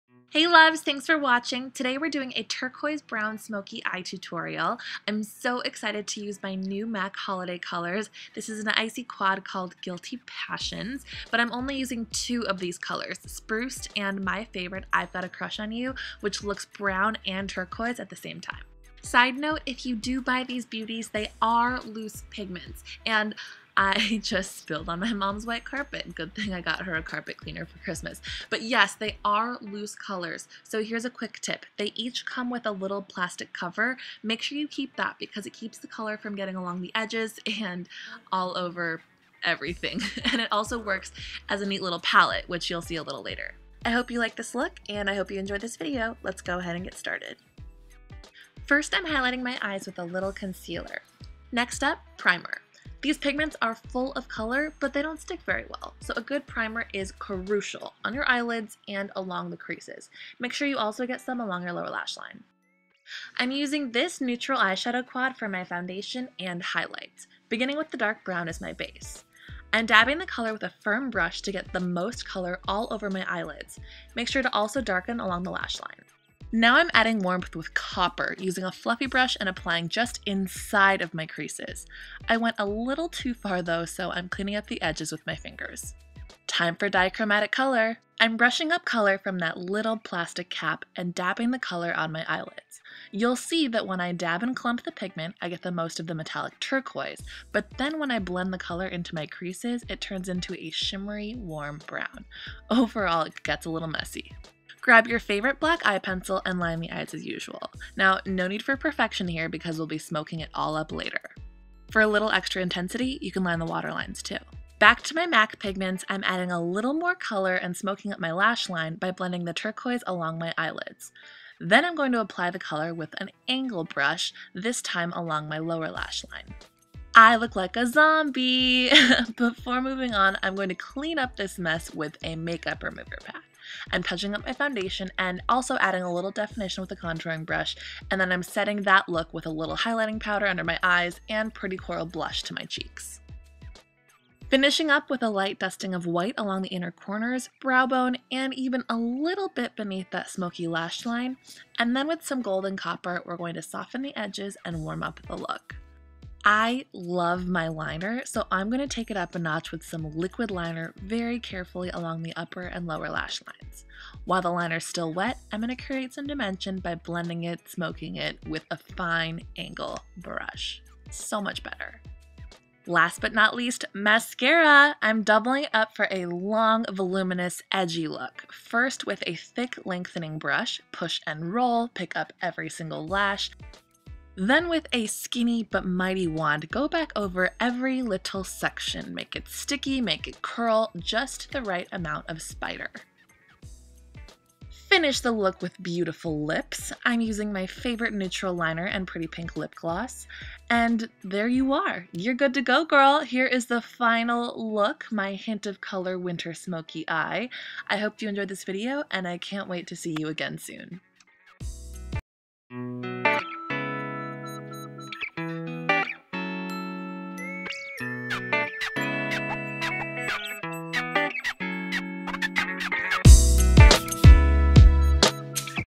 Winter Eye Makeup Tutorial with Turquoise Brown MAC Metallic Pigments
An edgy winter eye makeup tutorial, featuring the MAC "Guilty Passions" metallic pigments quad. Using two colors from the quad with some of my other favorite products, I've created a dichromatic turquoise brown smoky eye. I hope you like the look and enjoy the tutorial.

Products Used:
MAC "Guilty Passions" Metallic Pigments in "Spruced" and "I've Got a Crush on You"
COVERGIRL Eye Enhancers Quad in "Coffee Shop"
Make Up Forever HD Foundation
MAC Studio Fix Matte Foundation in C3
NARS (limited edition) Cheek Palette in "Foreplay"
YSL Lip Liner #13
MAC Cremesheen Glass in "Partial to Pink"

*Please notify via email before sending packages. There is no guarantee products will be received or used in future videos.